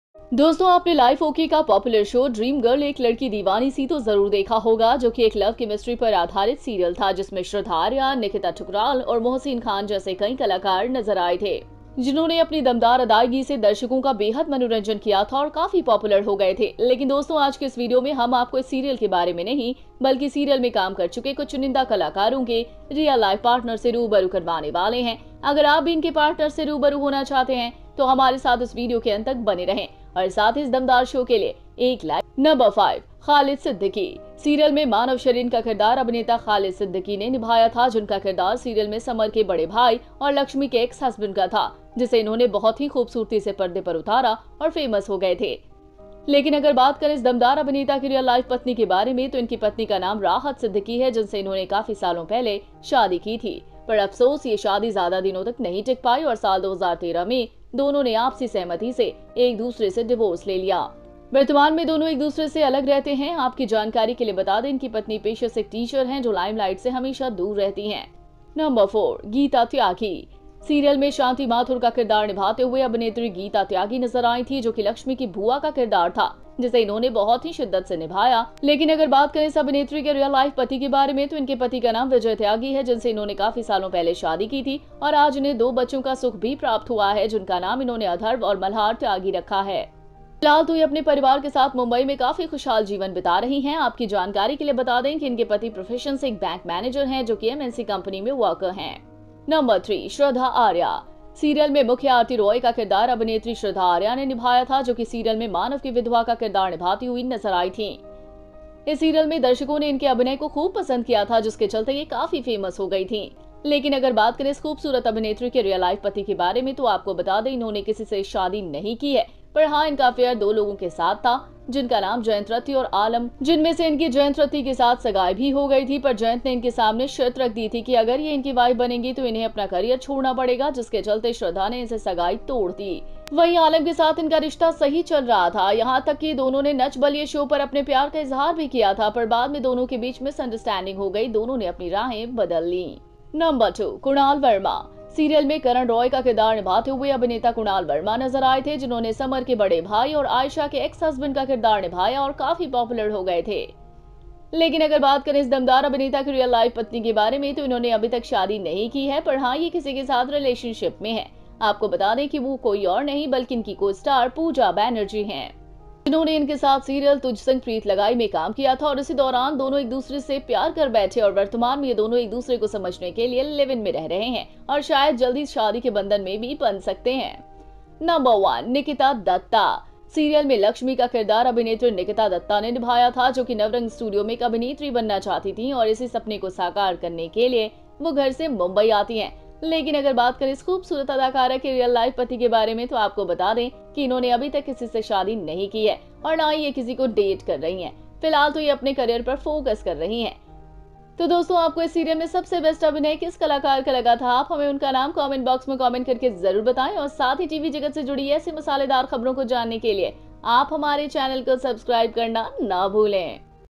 ड्रीम गर्ल सीरियल में काम करके प्रसिद्धि हासिल करने वाले कलाकारों के रियल लाइफ पार्टनर।
Title - Dream Girl A Girl Crazy!

 Friends must have seen the popular show "Dream Girl Ek Girl Deewani Si" from their Life OK, which was a love chemistry serial.
 In which many artists like Shraddha Arya, Nikita Thukral and Mohsin Khan were seen.

 Who entertained the audience very much with their strong payment and became quite popular.

 But friends, in this video of today, we are not going to tell you about this serial but in the serial, we are going to introduce you to the real life partner of selected artists.

 If you also want to get in touch with their partner then stay with us till the end of this video and at the same time it makes a like for this powerful show.

 Number - 5 Khalid Siddiqui

 In the serial, Manav Sharin was played by actor Khalid Siddiqui, whose character in the serial was Samar's elder brother and Lakshmi's X Husband, who had very beautifully come on screen and became famous.

 But if we talk about the real life wife of this powerful actor, then his wife's name is Rahat Siddhki whom he married many years ago, but now regret this marriage did not last long and in 2013, both of them agreed with each other  Took divors from each other.

 Currently, the two live separately from each other.  For your information, let us know that his wife is a teacher by profession, who is always away from limelight.

 Number 4 - Geeta Tyagi

 Actress Geeta Tyagi, who played the character of Shanti Mathur in the serial, was the character of Laxmi's aunt whom she played very well and became popular.

 But if we talk about the real life husband of this actress, then her husband's name is Vijay Tyagi whom she married many years ago and today she has got the happiness of two children whom she named Adharva and Malhar Tyagi.

 She is currently living a very happy life in Mumbai with her family.  For your information, let me tell you that her husband is a bank manager from the profession who is a worker in MNC company.

 Number 3 - Shraddha Arya

 In the serial, the lead role of Aarti Roy was played by actress Shraddha Arya who was seen playing the role of Manav's widow in the serial.

 In this serial, the audience also liked his acting very much, due to which it became quite famous.

 But if you talk about the real life husband of this beautiful actress, then tell you that she has not married anyone, but yes, she had an affair with two people named Jayant Ratti and Alam Mankar.

  Out of which he was also engaged to Jayant Ratti, but Jayant had placed a condition in front of him that if he would become his wife, he would have to give up his career, due to which Shraddha broke the engagement with him.

 Their relationship with the same Alam was going well, even both of them had expressed their love on the show "Nach Baliye" but later there was some misunderstanding between them that both of them changed their way.

 Number - 2 Kunal Verma

 Actor Kunal Verma was seen playing Karan Roy in the serial who played Sam's elder brother and Ayesha's X Husband and became quite popular.
 But if we talk about the real life wife of this powerful actor, then she has not married yet, but yes she is in a relationship with someone, let me tell you that she is none other than him, but her co-star Pooja Banerjee.  Who worked with them in the serial "Tujh Sang Preet Lagai", and at the same time the two fell in love with each other and are currently living in a live-in to understand each other, and maybe soon  Can also be tied in marriage.

 Number - 1 Nikita Dutta

 In the serial, Lakshmi was played by actress Nikita Dutta who wanted to become an actress in Navrang Studio and to come to Mumbai to realize this dream.  But if you talk about the real life husband of this beautiful actress, then tell you that she has not married anyone yet and neither is she dating anyone, at the moment it is engaged in focusing on her career.

 So friends, which artist did you think was the best acting in this serial?  You must tell us their name by commenting in the comment box and you should not forget to subscribe to our channel to know such spicy news related to TV world.